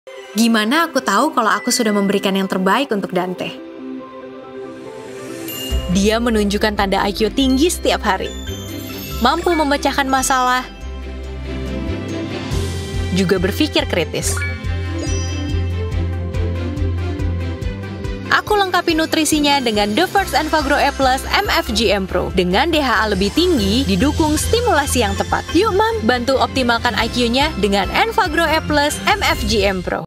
Enfagrow A PLus
How do you know that have given enough nutrition for your child? Chelsea Olivia shares her knowledge and how she ensures her child has the right nutrition for his growth.